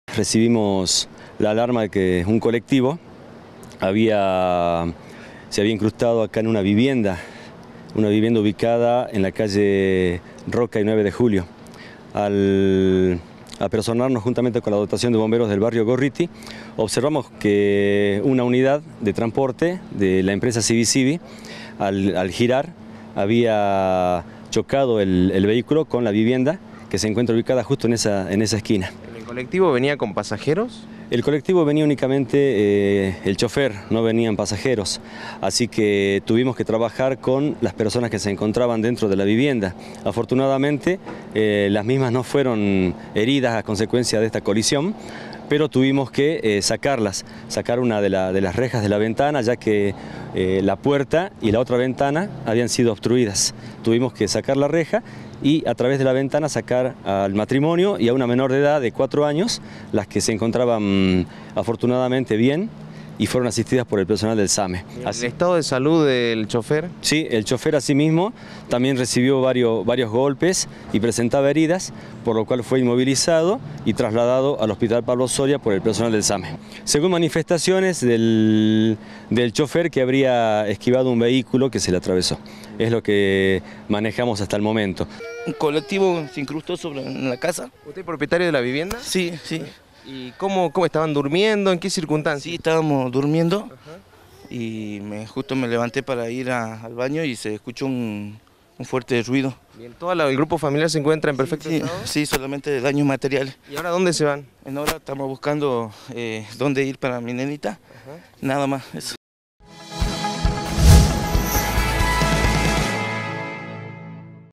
Un colectivo perdió el control y se incrustó en una casa de barrio Almirante Brown
El chofer sufrió golpes y heridas por lo que tuvo que ser trasladado al hospital Pablo Soria.